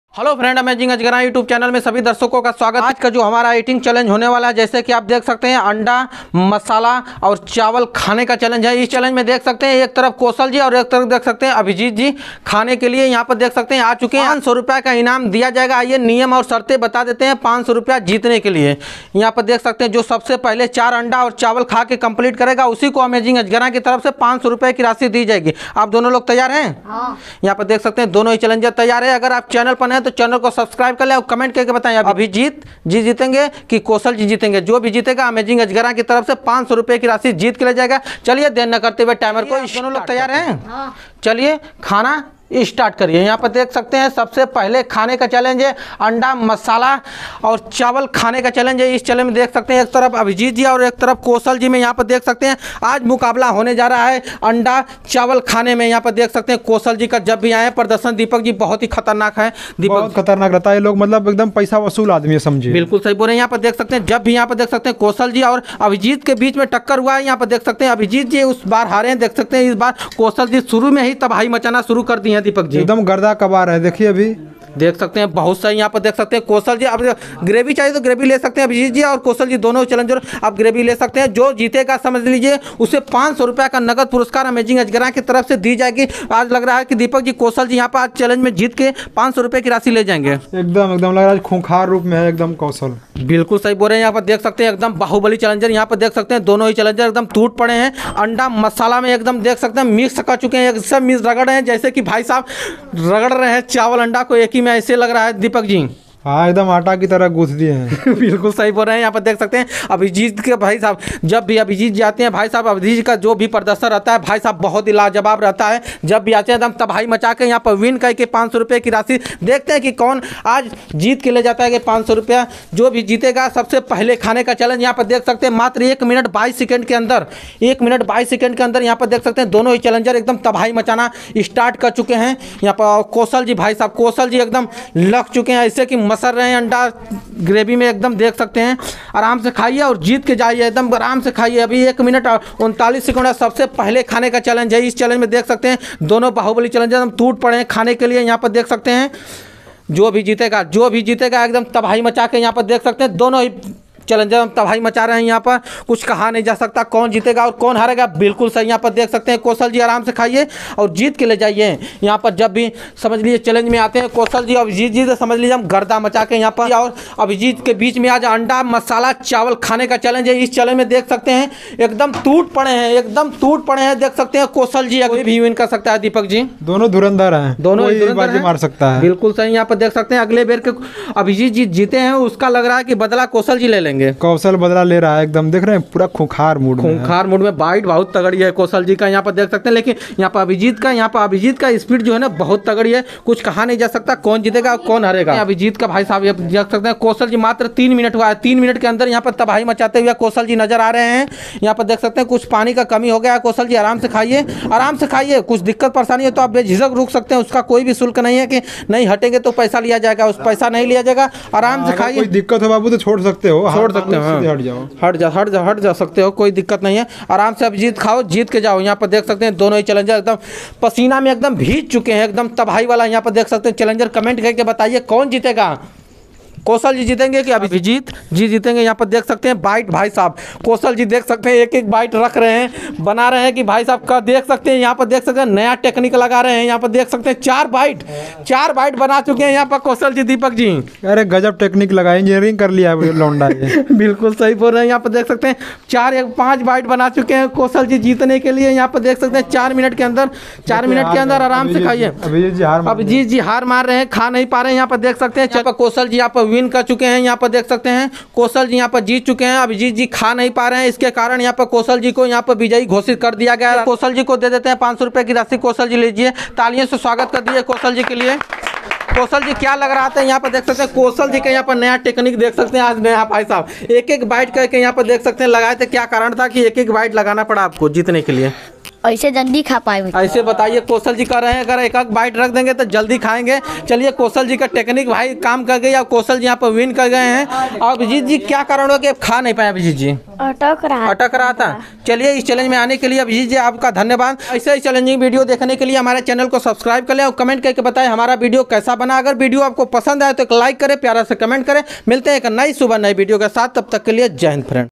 अंडा🥚मसाला चावल 🍚 सबसे पहले खाओ ₹500💲ले जाओ || Egg 🥚 curry rice 🍚🌾 eating and win ₹500 inam lejao😱👍🙏
अंडा 🥚 मसाला चावल 🍚 सबसे पहले खाओ ₹500💲💰💸 inam lejao 😱 || Egg 🥚 curry rice 🍚🌾 eating and win ₹500 💲💰💸 ||  amazing ajagara 😱🤣🤩🙏👍